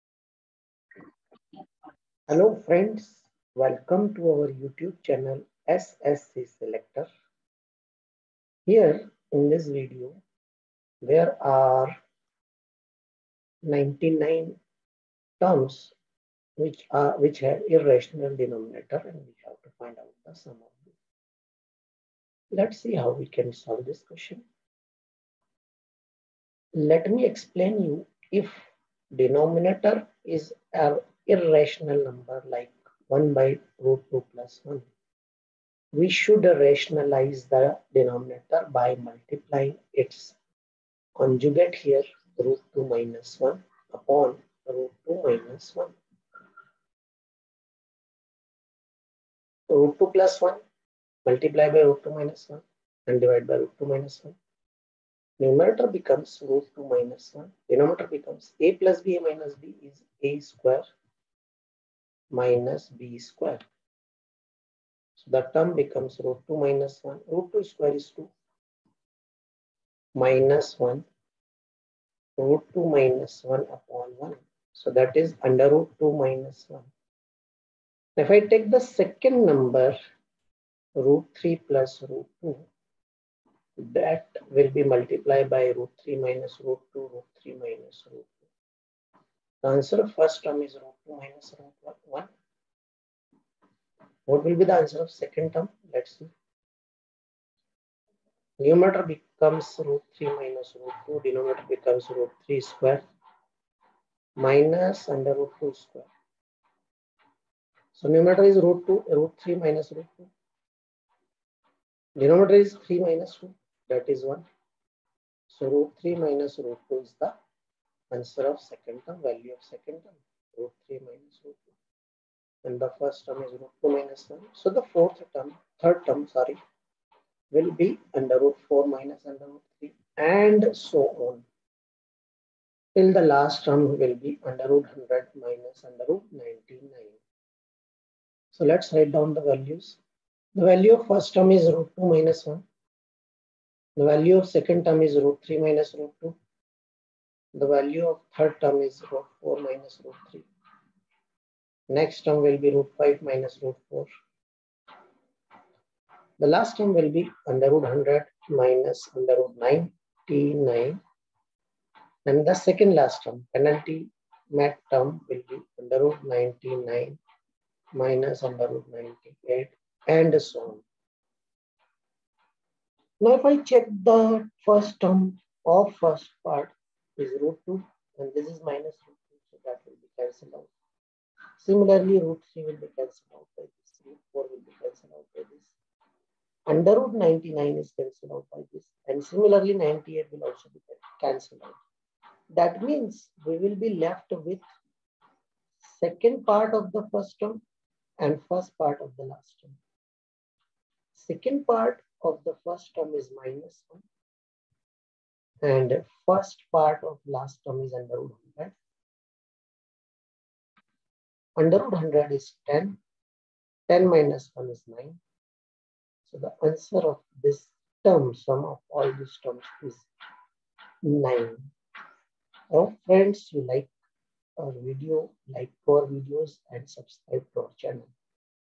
Find the value of 1/((sqrt(2) + sqrt(1)) + 1/((sqrt(3) + sqrt(2)) + 1/((sqrt(4) + sqrt(3)) .........
Find the value of 1/((sqrt(2) + sqrt(1)) + 1/((sqrt(3) + sqrt(2)) + 1/((sqrt(4) + sqrt(3)) + 1/((sqrt(100) + sqrt(99))